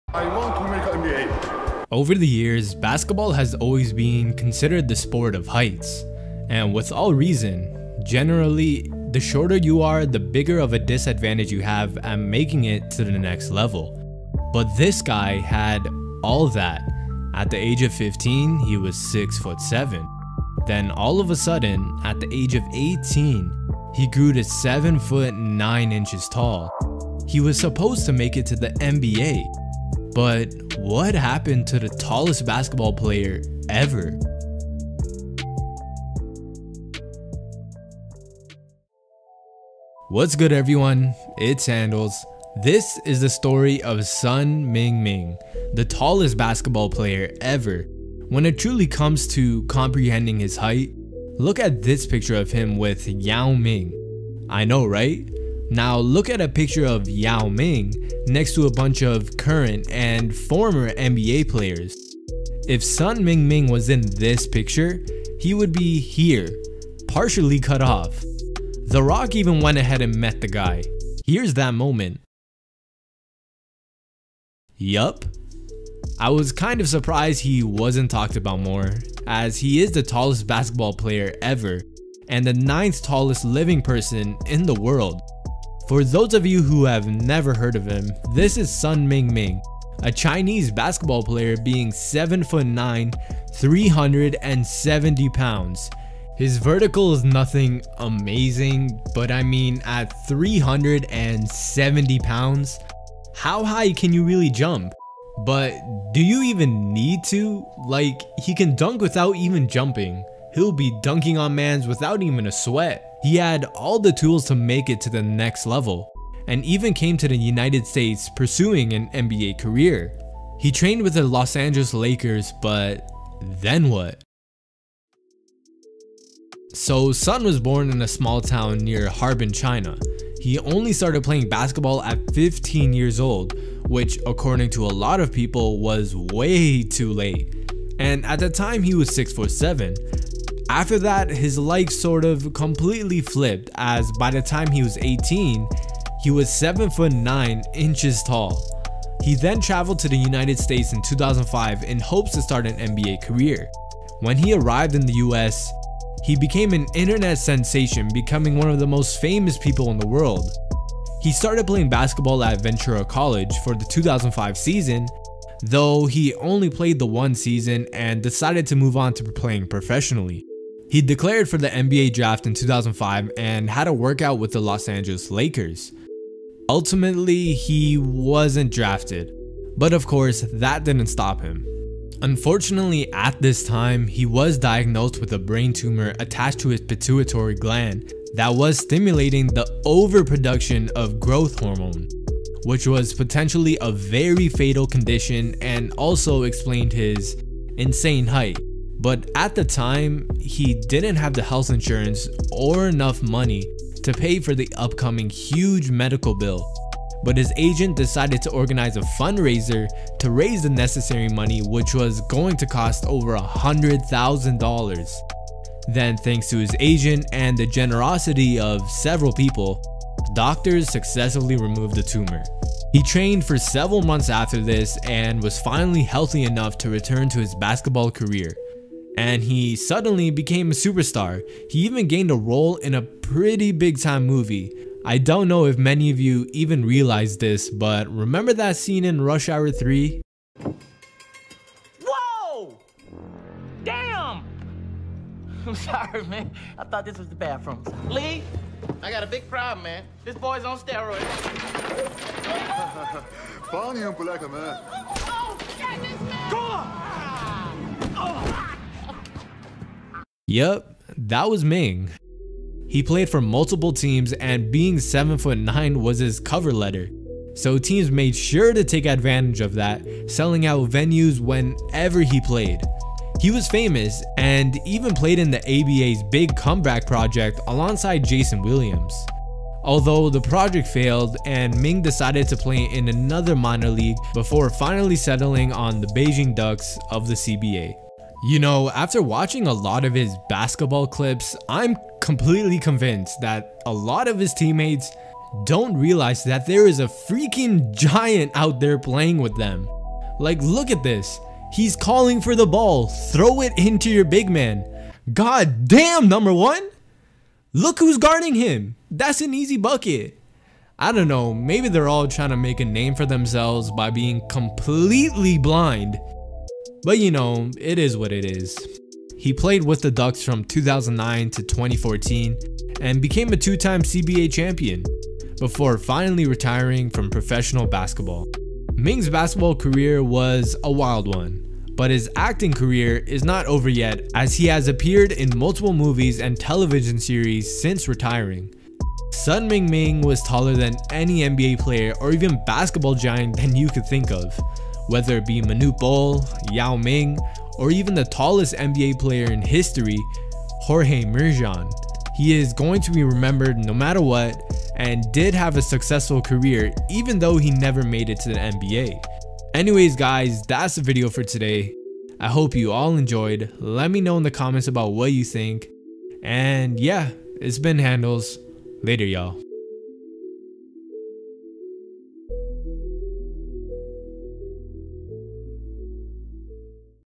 The TALLEST Basketball Player EVER That Was TOO TALL For The NBA
He was a celebrity, said to go to the NBA. Just what happened to the tallest basketball player ever? His name was Sun Ming Ming